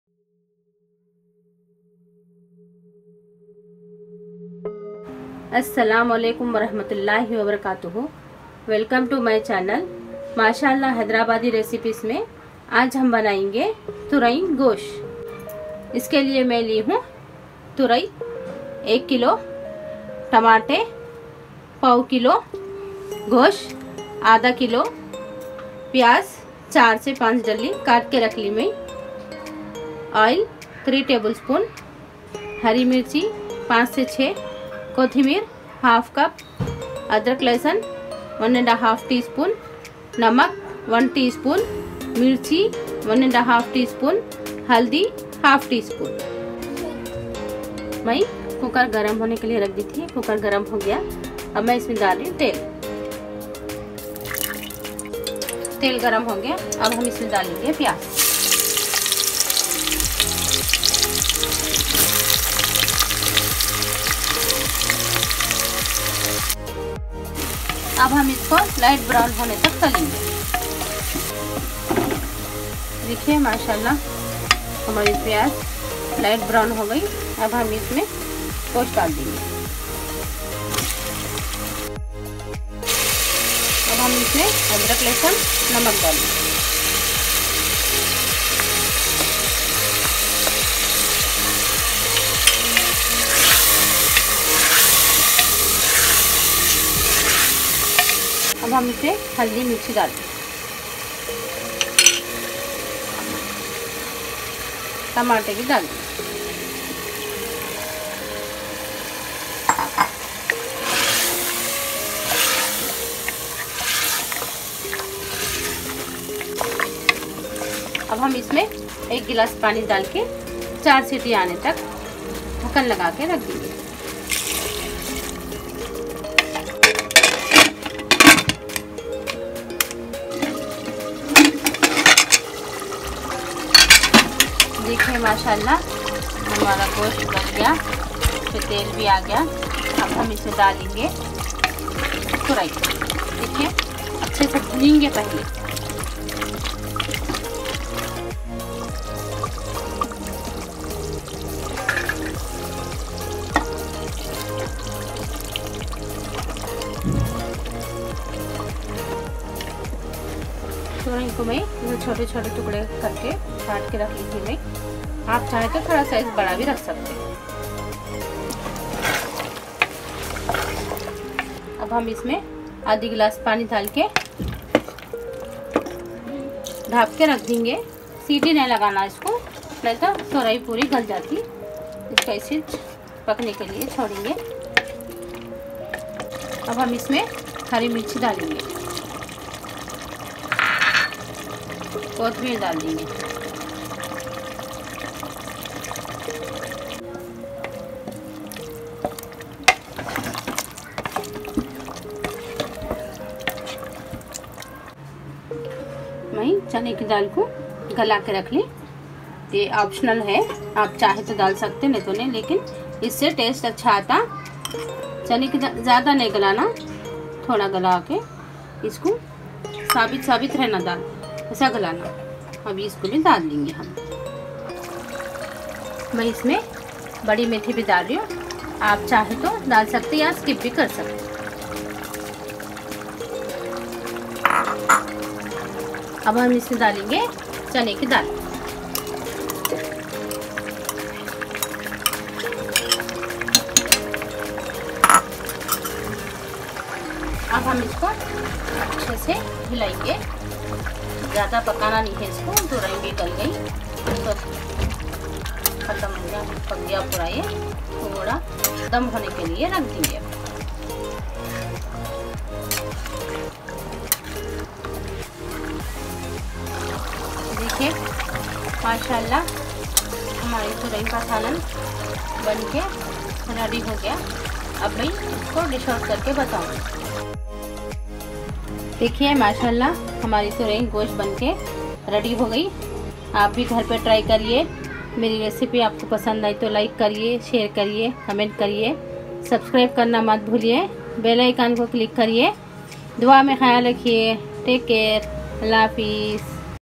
Hyderabadi Turai gosh😋 | Mashallah hyderabadi recipes
Ingredients for Turai Gosh -

Turai :- 1kg
Tomato :- ¼kg
Gosh :- ½ kg
Onion :- 4-5
Kothimir :- ½ cup

Namak :- 1 teaspoon
Lal mirch powder :- 1½ teaspoon
Haldi :- ½ teaspoon
Adrak laisan :- 1½ teaspoon